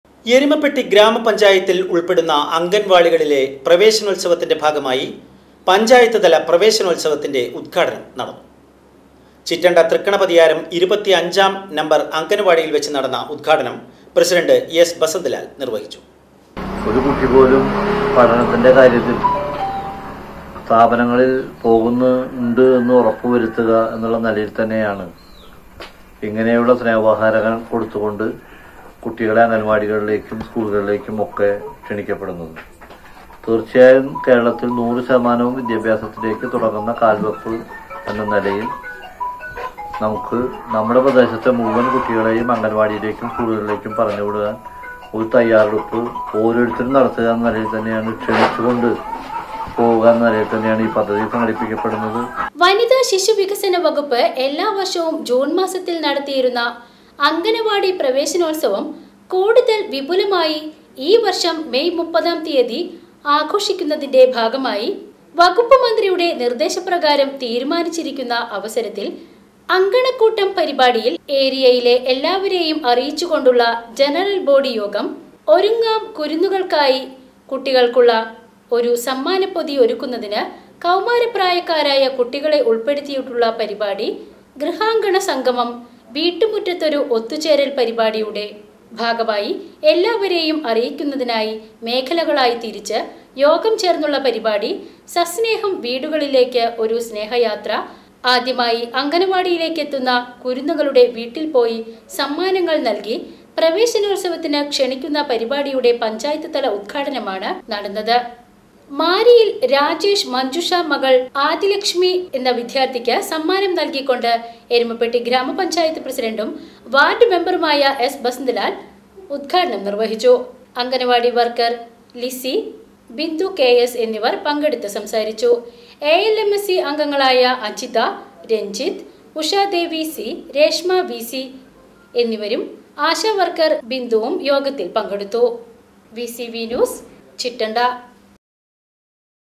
എരുമപ്പെട്ടി പഞ്ചായത്തിൽ അംഗനവാടി പ്രവേശനോത്സവത്തിന്റെ ഭാഗമായി പഞ്ചായത്ത് തല ഉദ്ഘാടനം നടന്നു
NEWS UPDATES -25 - MAY -2023

VCV NEWS

Wadakkanchery, Thrissur